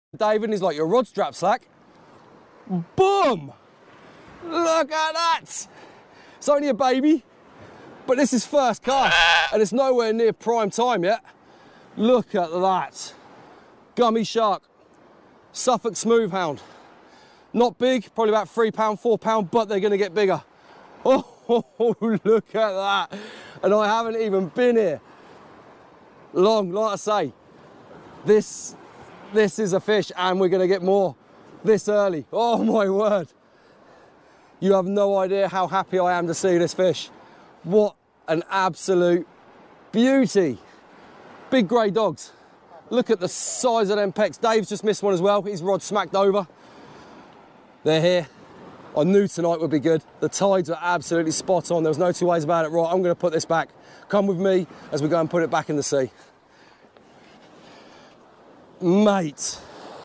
Catching A Gummy Shark From A UK Beach
A smooth hound shark caught from a Suffolk beach on a recent trip.